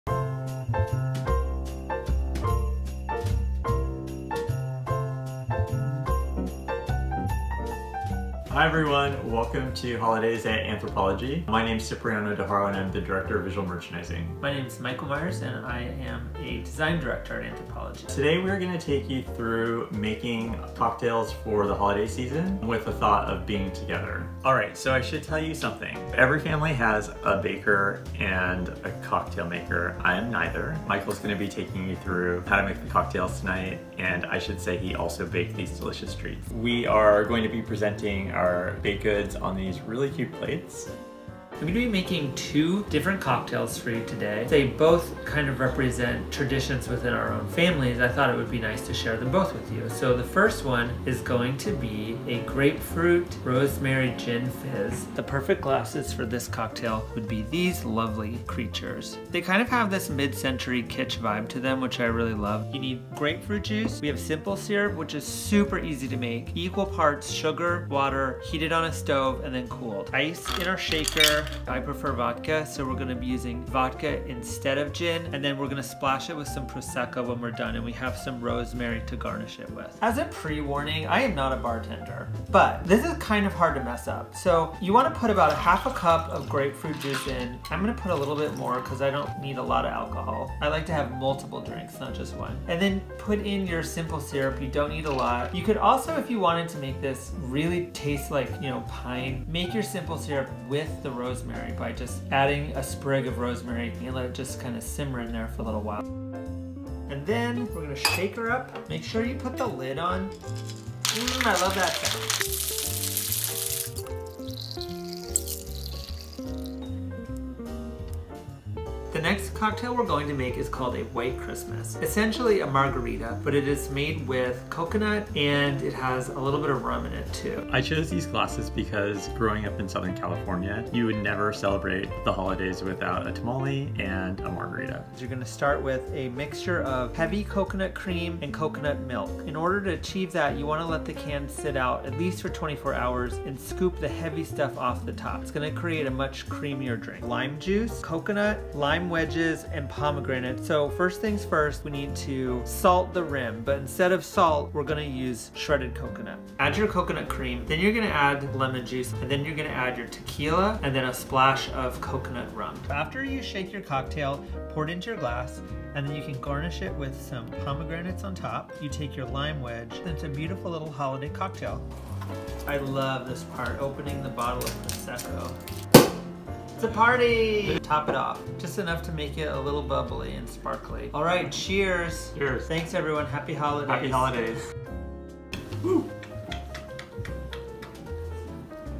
Our 2 Favorite Gin & Tequila-Based Holiday Cocktails
Holiday Spirits · Shake up a holiday gin cocktail and a Christmas tequila cocktail with Design Director Michael Meyers and Director of Visual Merchandising Cipriano De Haro 🍾